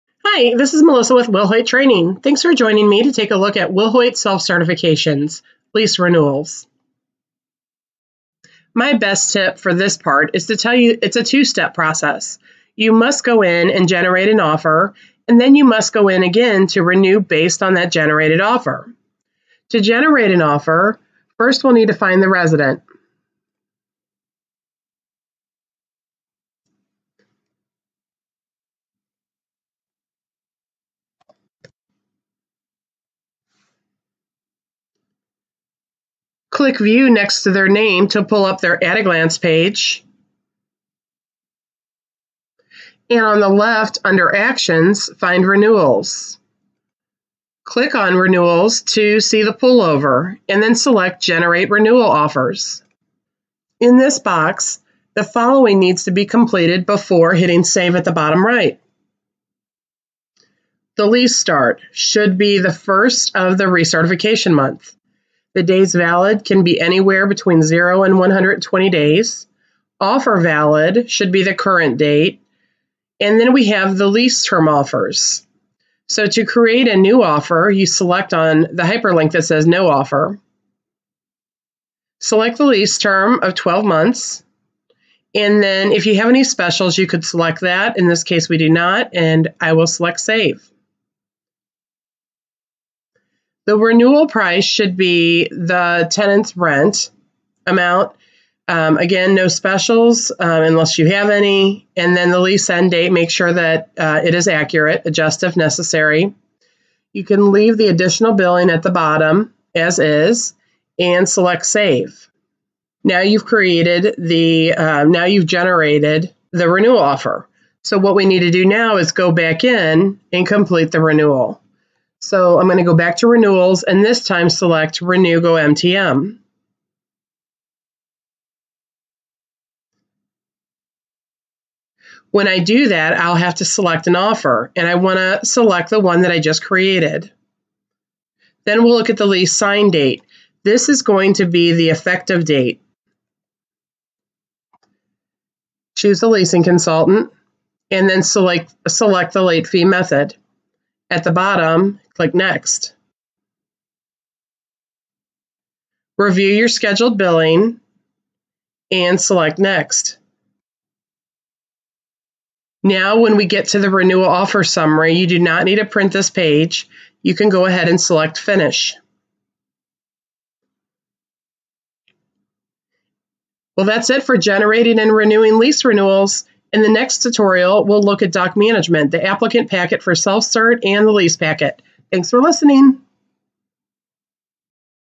Self Certs: Lease Renewals
WPI Self Certification Process in OneSite
This is Step 3 in our Self Certification process in OneSite.
Step 1 – Update Member Information and Financial Items
Step 2 – Run Trial Certification
Step 3 – Lease Renewals
Step 4 – Document Management: Applicant Packet for Self Cert and Lease Packet
Step 5 – Upload Documents to Packets and Case Manager
Step 6 – Complete and File